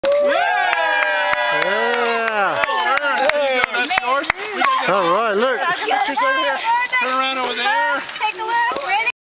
Lukes Game Ball
Luke getting his first game ball for pitching 2 innings (with a couple of strike outs) and 2 home runs!